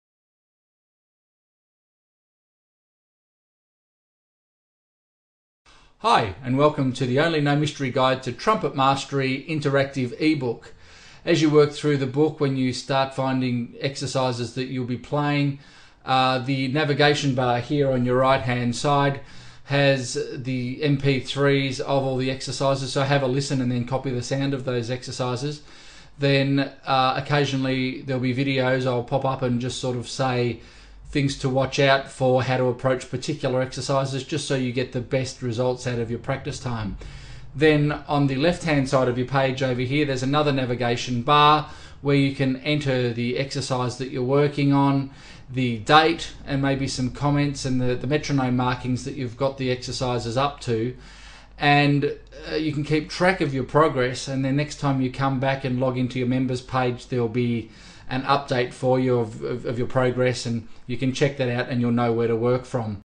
eBook Welcome SAMPLE
A sample of the Mystery to Mastery "Only No Mystery Guide to Trumpet Mastery" eBook welcome page